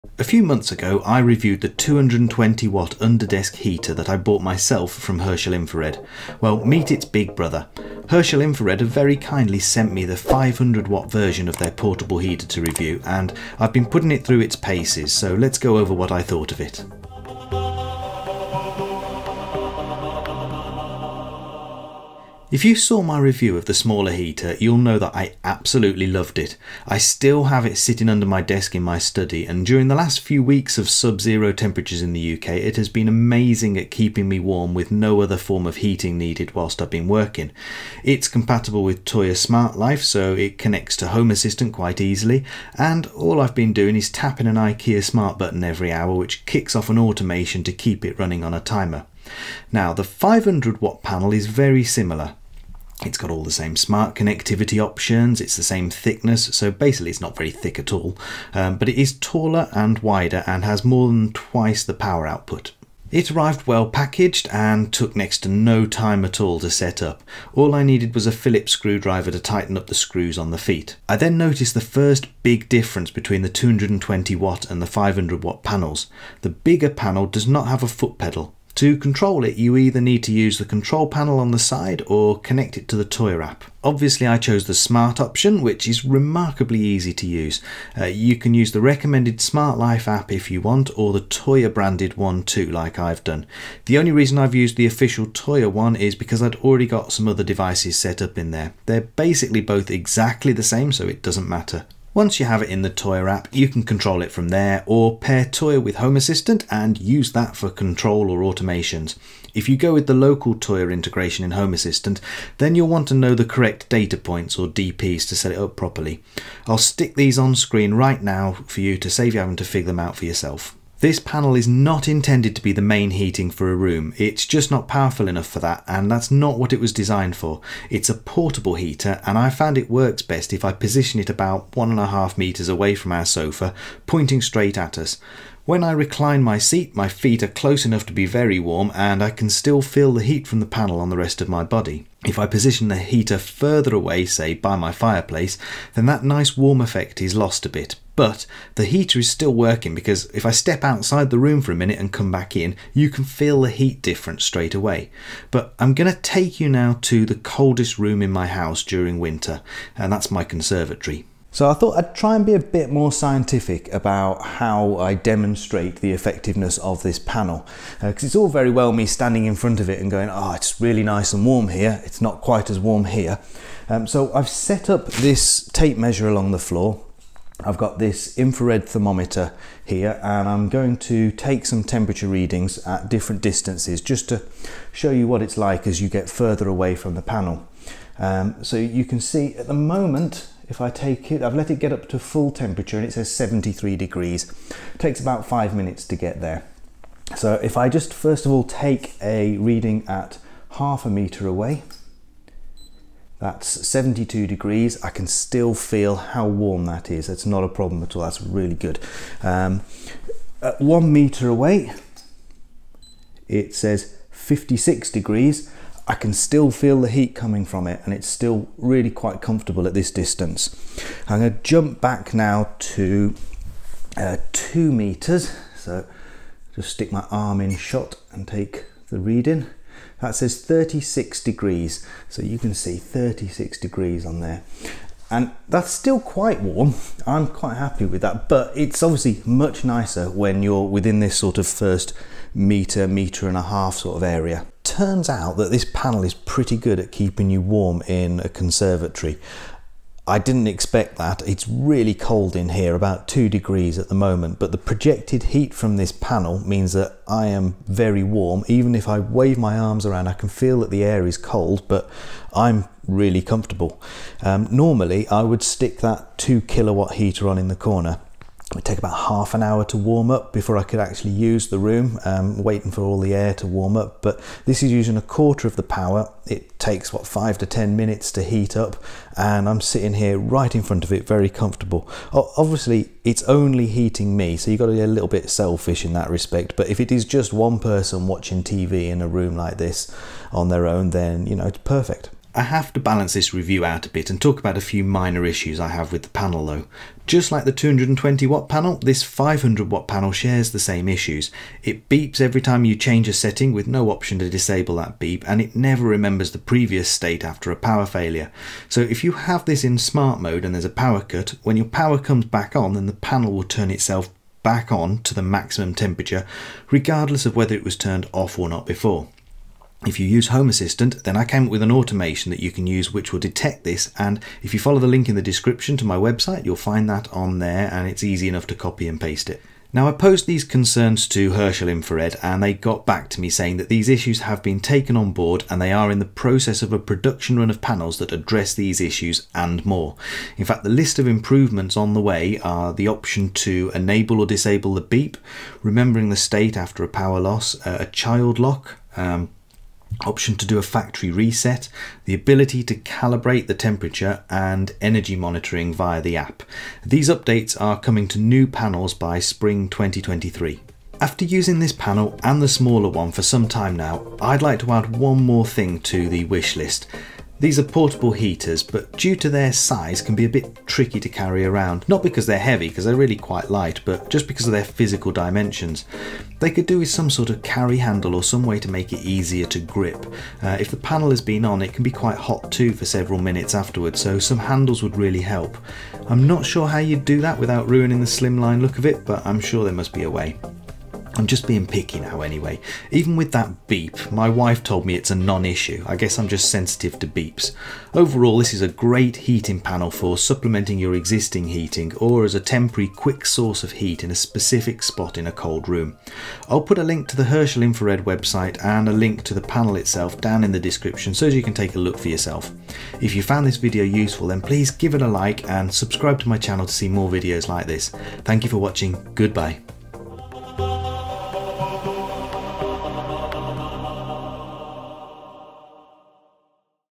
Herschel Infrared 500W Smart Portable Panel Heater Review
Herschel Infrared sent me their 500W smart wi-fi connected portable heater panel (HS500PR) to review. This review is my own independent opinion and I go over features, effectiveness, and compare it to the smaller 220W infrared panel I reviewed previously.